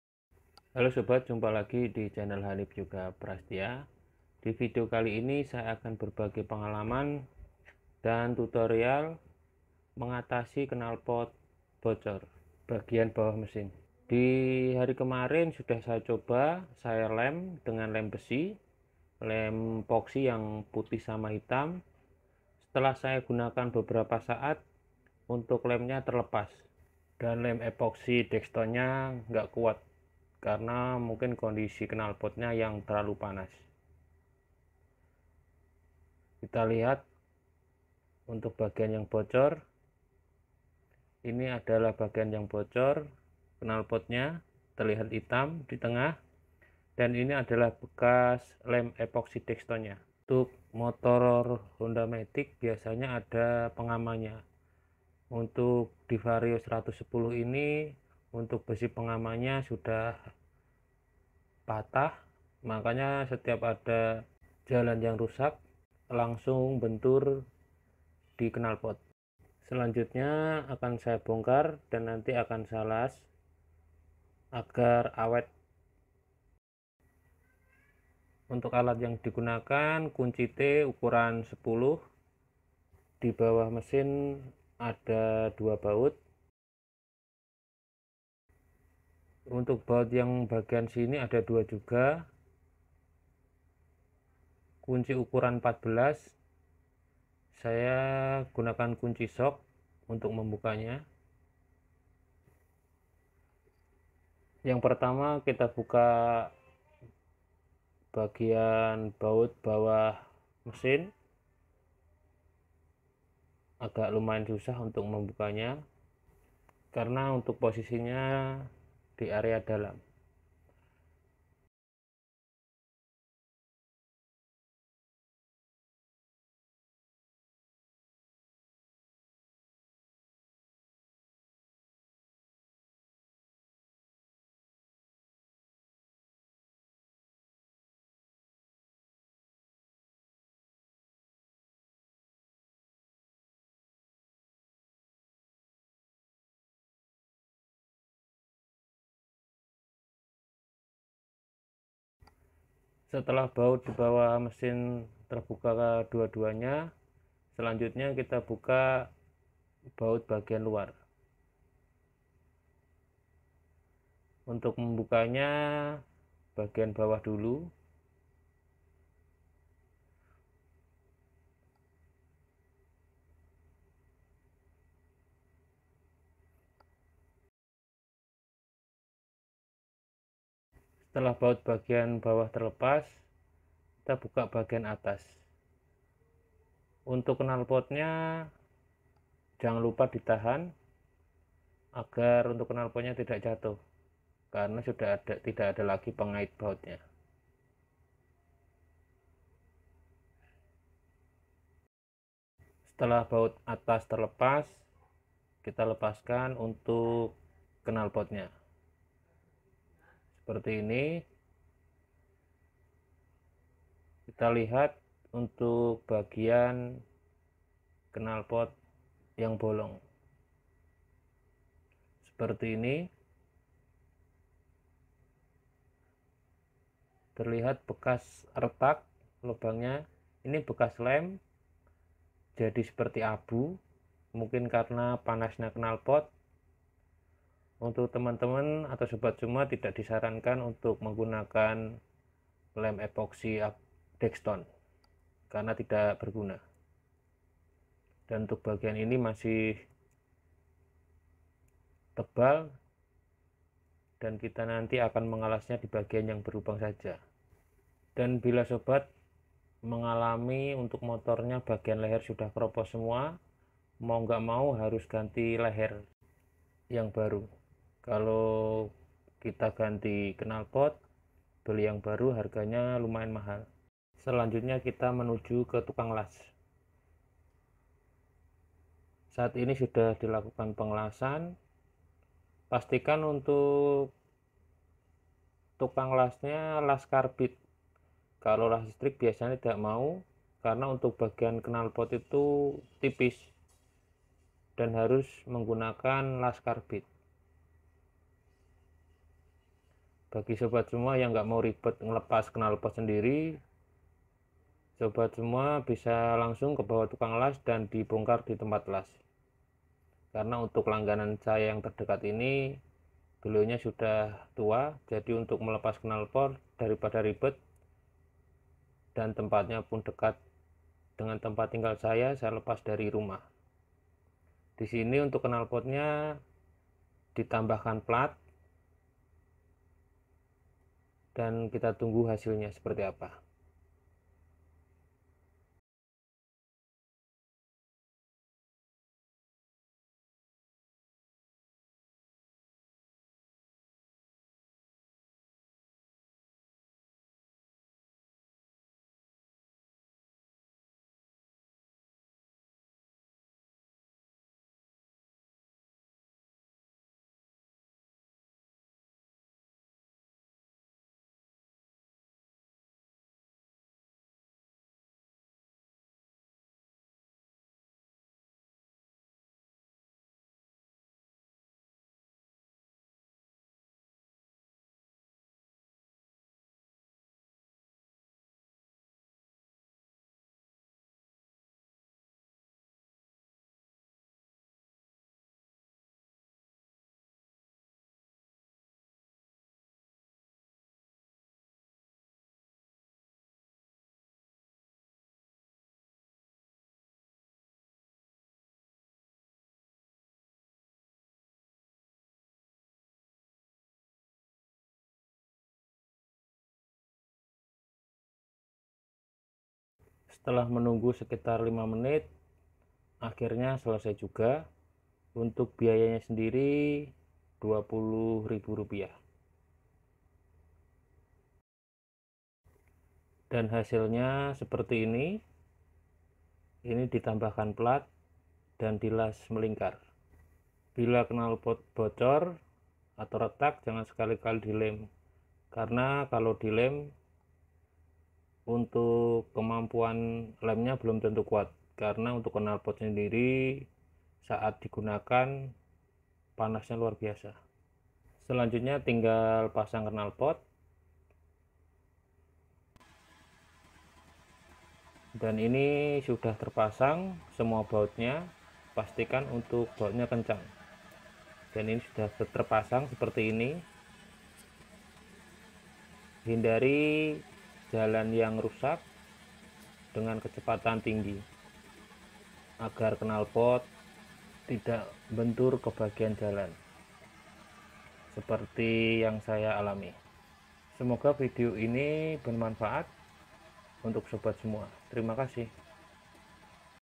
Cara mengatasi knalpot retak/bocor di motor vario 110
Cara mengatasi knalpot retak atau bocor pada motor matic honda vario 110 jangan gunakan lem untuk menambalnya karena knalpot saat kondisi motor di gunakan suhunya sangat panas lem pun akan lepas.pastikan di las agar awet kecuali kerusakan pada knalpot yang keropos harus ganti leher atau ganti yang baru dilihat dari kondisi kerusakannya